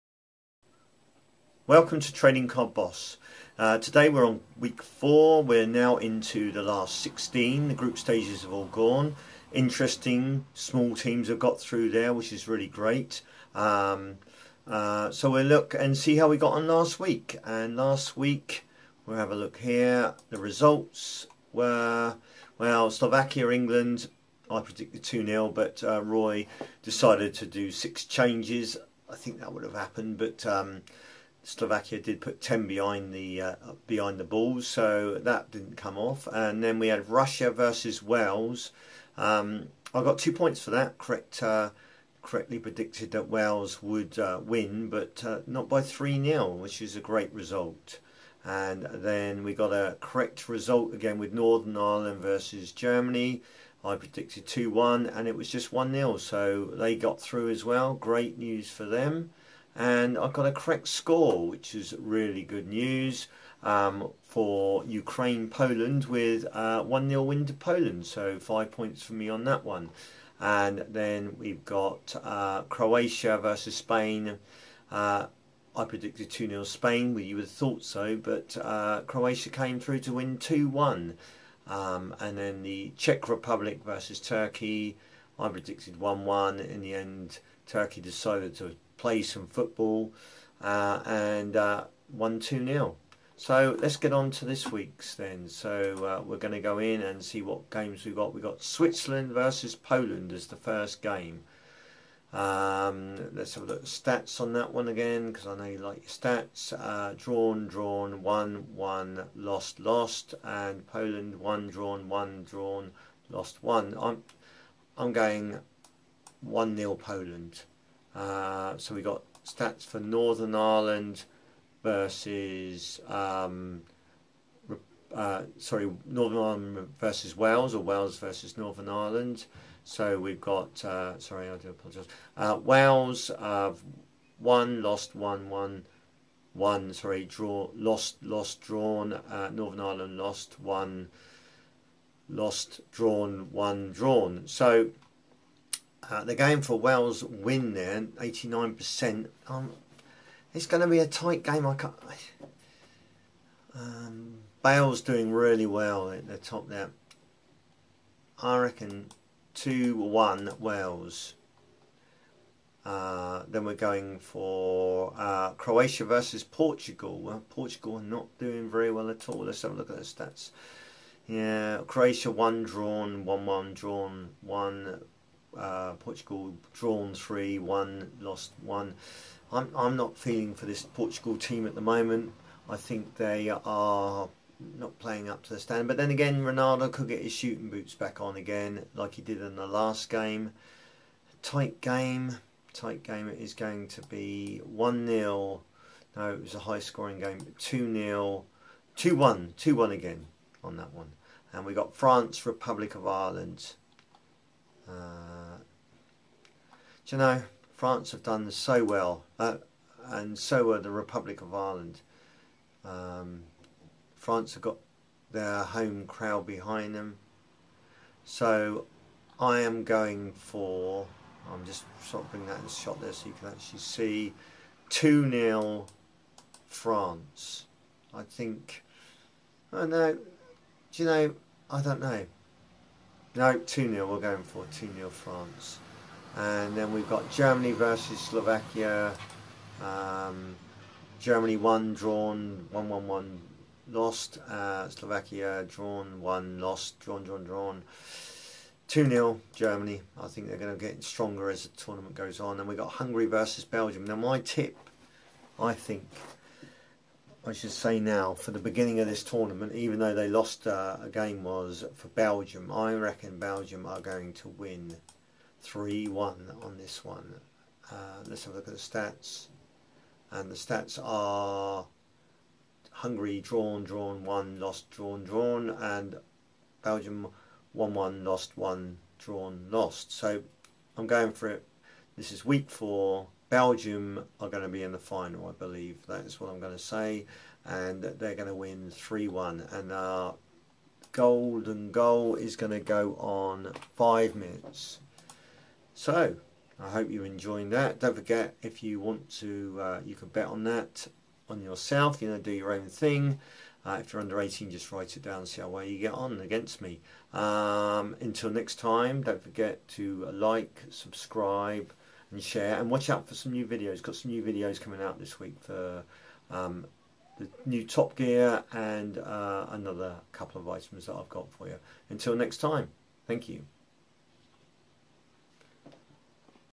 Euro 2016 France predictions week 4 from 23rd July 2016
My predictions for week 4 of the euro's this is now the last 16 of the competition with lots of small teams still in the tournament .